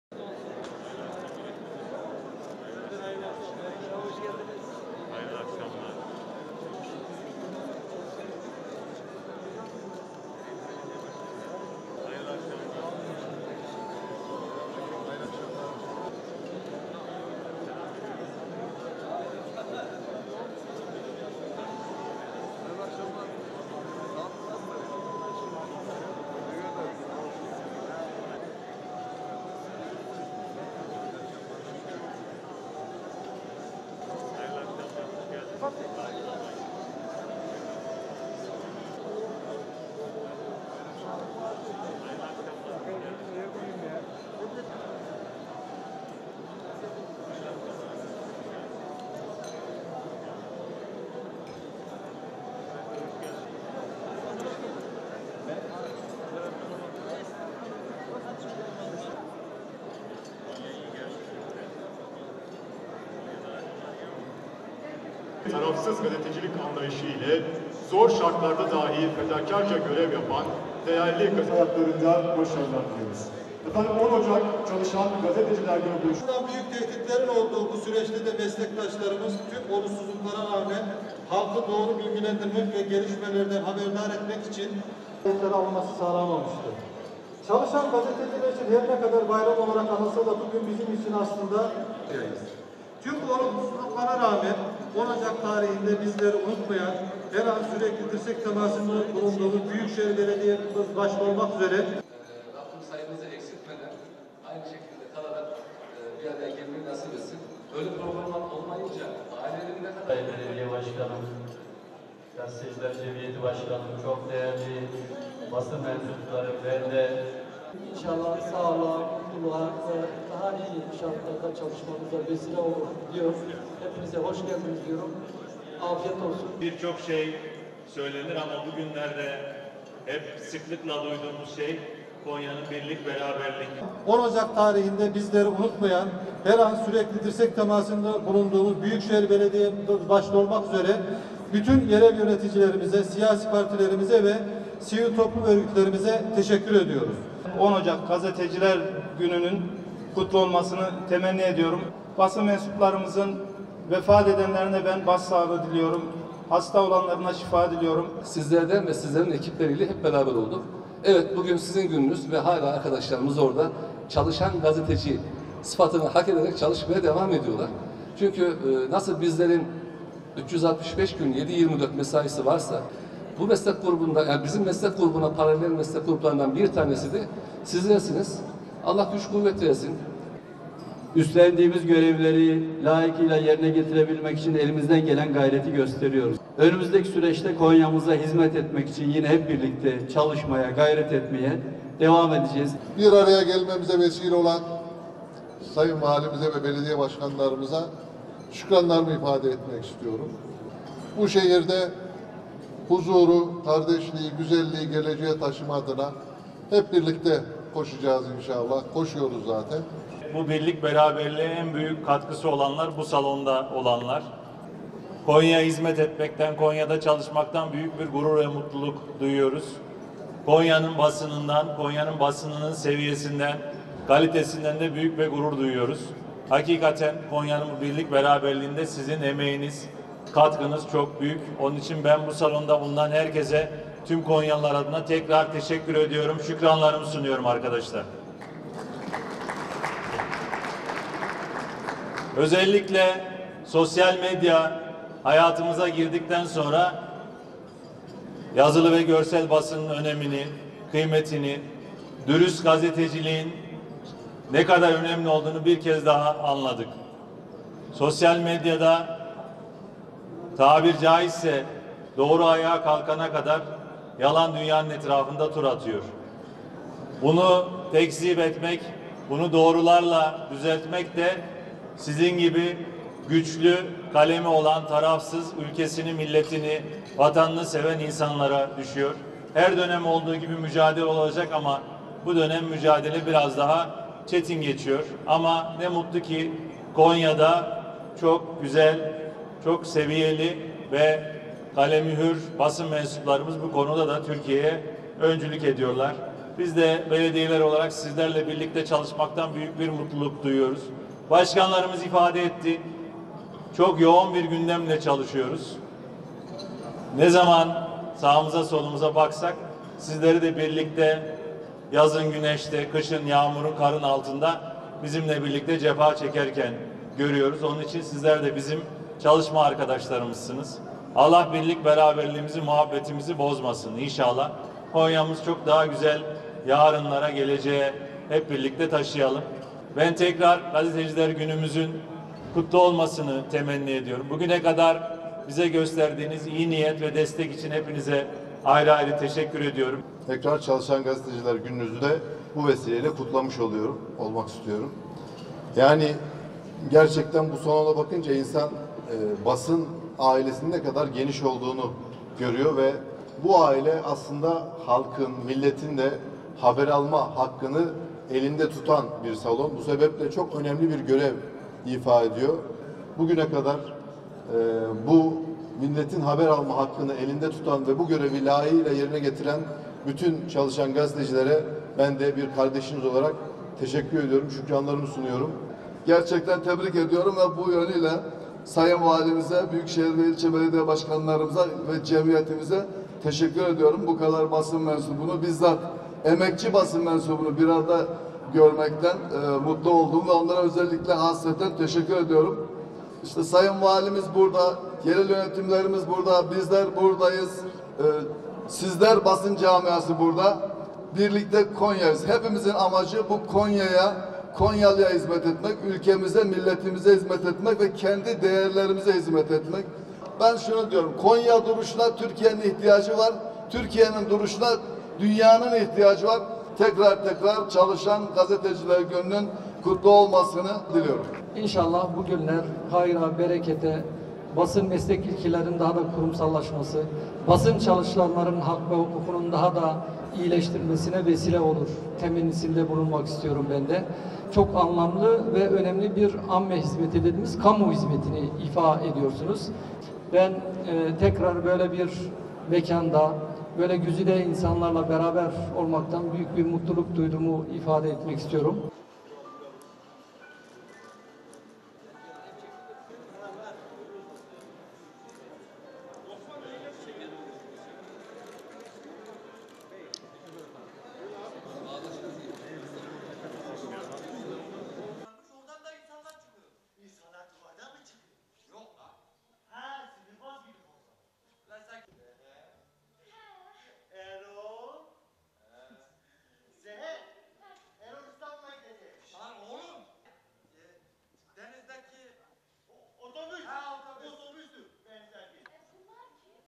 “Konya Basınıyla Gurur Duyuyoruz”
Konya Valisi Vahdettin Özkan, AK Parti Konya Milletvekilleri Ahmet Sorgun ve Selman Özboyacı, Konya Büyükşehir Belediye Başkanı Uğur İbrahim Altay, AK Parti Konya İl Başkanı Hasan Angı ile Karatay Belediye Başkanı Hasan Kılca, Meram Belediye Başkanı Mustafa Kavuş ve Selçuklu Belediye Başkanı Ahmet Pekyatırmacı, 10 Ocak Çalışan Gazeteciler Günü’nde Konya’nın 31 ilçesindeki basın-yayın kuruluşlarının çalışanlarıyla bir araya geldi. Konya Büyükşehir Belediye Başkanı Uğur İbrahim Altay, Konya’daki birlik ve beraberlik duygusunun çok yüksek olduğunu ifade ederek, “Konya basınının seviyesinden ve kalitesinden büyük gurur duyuyoruz. Konya’nın bu birlik ve beraberliğinde sizin emeğiniz, katkınız çok büyük. Onun için bu salonda bulunan herkese tüm Konyalılar adına şükranlarımı sunuyorum.” dedi.

Konya Valiliği, Konya Büyükşehir Belediyesi ve Selçuklu, Meram, Karatay Belediyeleri, 10 Ocak Çalışan Gazeteciler Günü dolayısıyla şehir genelinde faaliyet gösteren basın-yayın kuruluşlarının yöneticilerini ve çalışanlarını bir araya getirdi. Selçuklu Kongre Merkezi’nde gerçekleştirilen programda konuşan Konya Gazeteciler Cemiyeti Başkanı Sefa Özdemir, 10 Ocak’ta kendilerini unutmayan Büyükşehir Belediyesi başta olmak üzere bütün yerel yöneticilere, siyasi partilere ve sivil toplum örgütlerine teşekkür etti. Karatay Belediye Başkanı Hasan Kılca, tüm gazetecilerin 10 Ocak Çalışan Gazeteciler Günü’nü kutlayarak, basın camiasında vefat edenlere Allah’tan rahmet, hasta olanlara şifa diledi. Meram Belediye Başkanı Mustafa Kavuş, “Her zaman sizlerle ve sizlerin ekipleriyle beraber olduk. Nasıl bizlerin 365 gün 24 saat mesaisi varsa bizim meslek grubuna paralel meslek gruplarından bir tanesi de sizlersiniz. Allah güç kuvvet versin.” diye konuştu. Selçuklu Belediye Başkanı Ahmet Pekyatırmacı, “Üstlendiğimiz görevleri layıkıyla yerine getirebilmek için elimizden gelen gayreti gösteriyoruz. İnşallah önümüzdeki süreçte Konya’mıza hizmet etmek için hep birlikte çalışmaya devam edeceğiz.” dedi. AK Parti Konya İl Başkanı Hasan Angı, “Bir araya gelmemize vesile olan Sayın Valimize, belediye başkanlarımıza şükranlarımı sunuyorum. Bu şehirde; huzuru, kardeşliği, güzelliği yarınlara taşıma adına hep birlikte koşuyoruz.” ifadelerini kullandı.
KONYA’NIN BASININA TEŞEKKÜR EDİYORUZ
Konya Büyükşehir Belediye Başkanı Uğur İbrahim Altay, Konya’daki birlik ve beraberlik duygusuna vurgu yaparak konuşmasına başladı. Başkan Altay, “Buna en büyük katkısı olanlar bu salonda olanlar. Konya’ya hizmet etmekten büyük gurur duyuyoruz. Konya basınının seviyesinden ve kalitesinden büyük gurur duyuyoruz. Konya’nın bu birlik ve beraberliğinde sizin emeğiniz, katkınız çok büyük. Onun için bu salonda bulunan herkese tüm Konyalılar adına şükranlarımı sunuyorum.” açıklamasını yaptı.